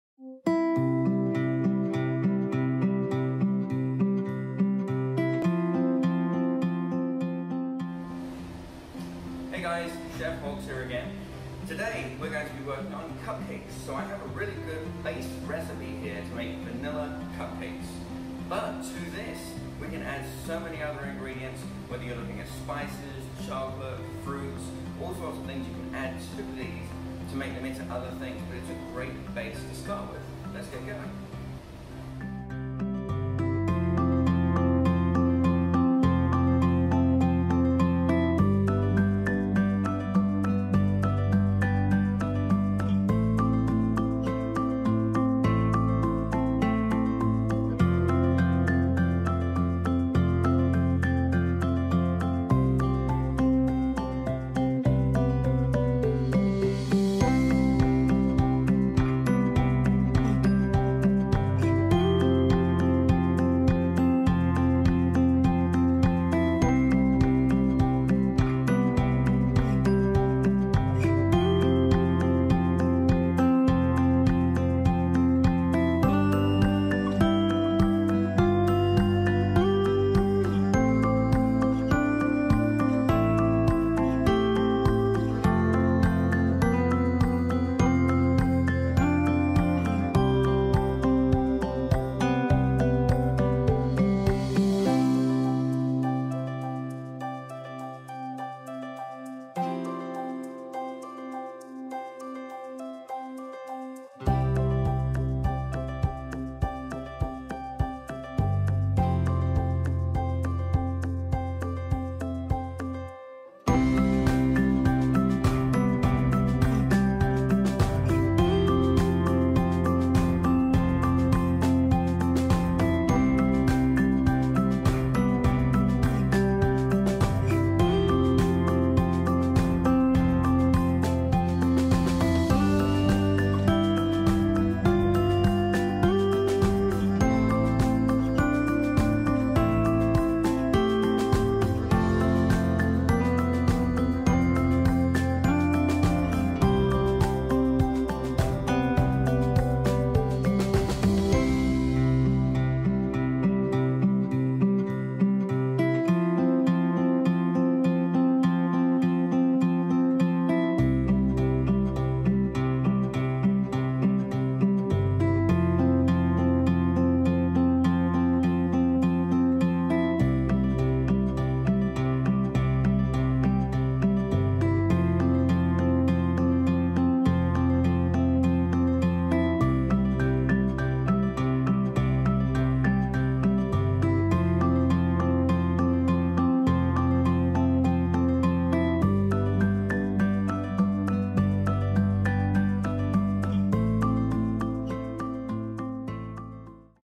Easy to make - Base For Every Cupcake
Ingredients:
225g sugar
225g butter (softened)
4 eggs
200g all purpose flour
25g Corn flour /starch
5ml (TSP) baking powder
30ml (TBSP) milk
15ml (TBSP) Vanilla extract